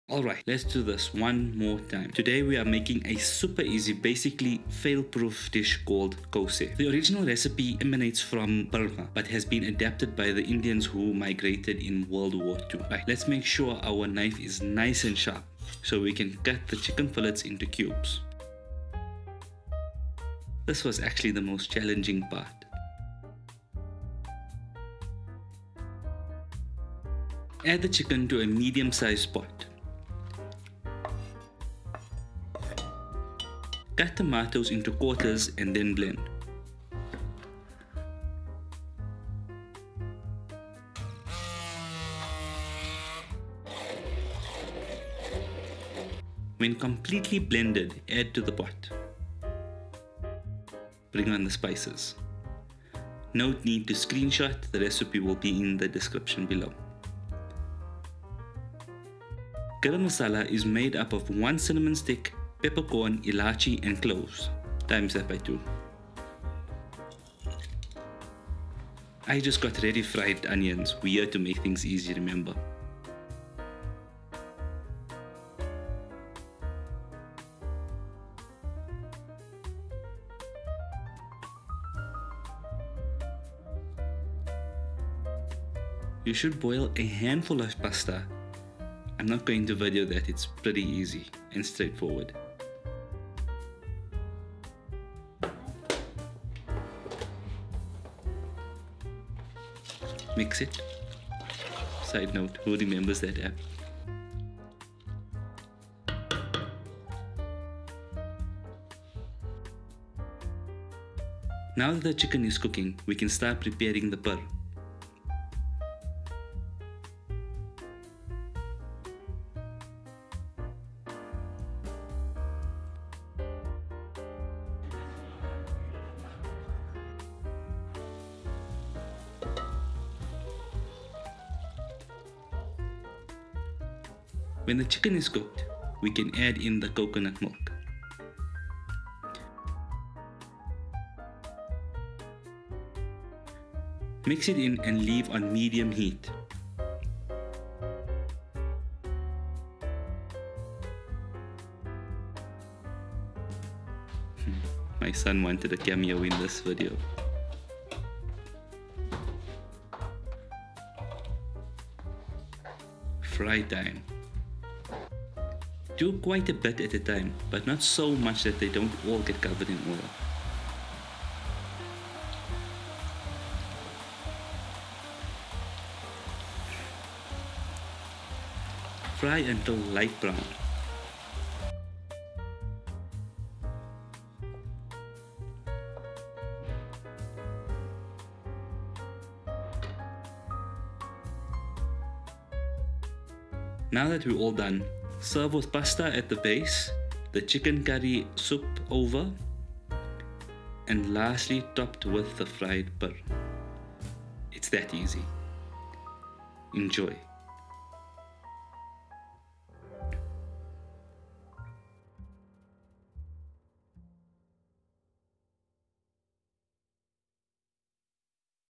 Khowse (khow suey) | Indian Style | Pasta Chicken Dish
Super easy and delicious dish to make. The effort : reward ratio is definitely worth it. Also it would have taken much less time to do if I wasn’t filming as well. Khowse is a pasta based dish with chicken currry and pur topping. Warning you’ll end up picking on the pur even after the dish is done.

Recipe:
0.5kg Chicken (cubbed)
1t Salt
Handful Fried Onions
1/2t Dana
1/2t Jheero
1t ginger Garlic Masala
1/2t Green Masala
1 Tomato
1/2t Arad
2 Garam Masala
1T Lemon Juice

Cook then add
1 tin Coconut Milk

2 handfuls of spaghetti
1t Salt

t = Teaspoon
T = Tablespoon